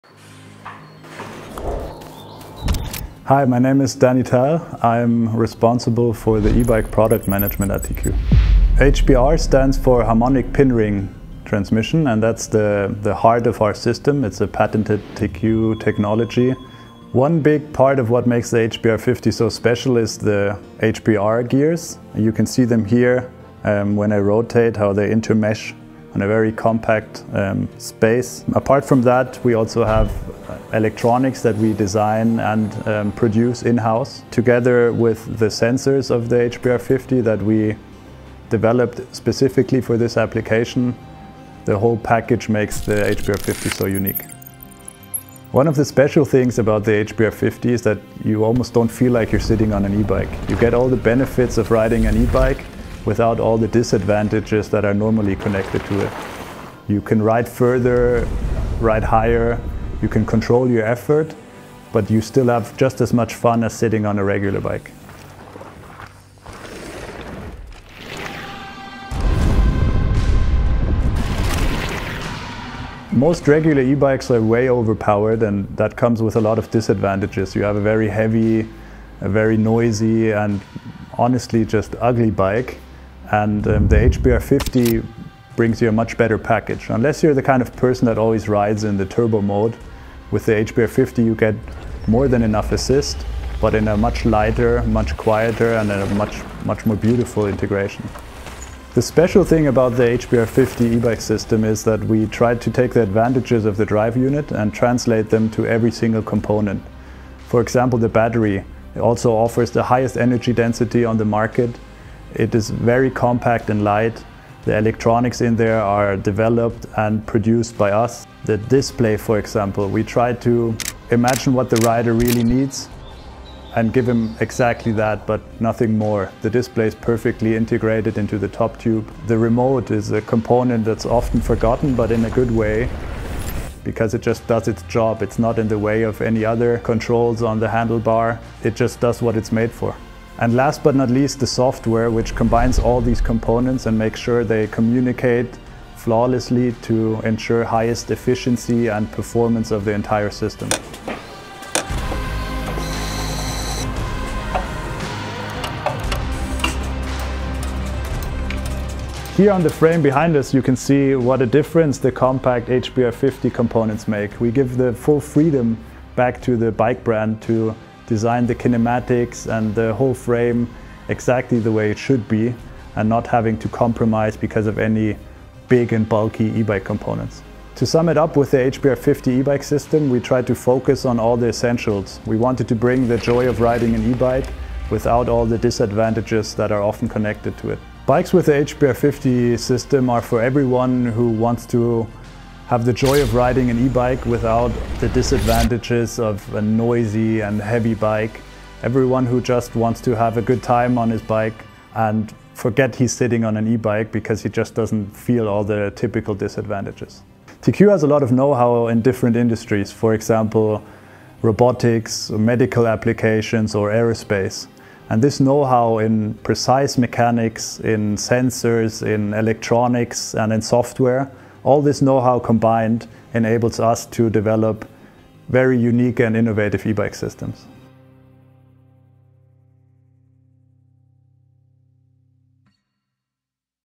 Inside the TQ-HPR50 – Everything You Need to Know About The most Precise E-Bike Motor!
The TQ-HPR50 motor is revolutionizing the E-Bike world with its ultra-compact design, precise performance, and impressive efficiency. But what makes it so special?
In this video, Product Manager Daniel Theil explains everything about this innovative drive system, including its technical details, advantages, and why it’s the best choice for MTB, road, or gravel riders.

🔋 Topics covered in this video:
✅ How the HPR50 motor works
✅ Advantages over other E-Bike motors
✅ Who is the TQ-HPR50 best suited for?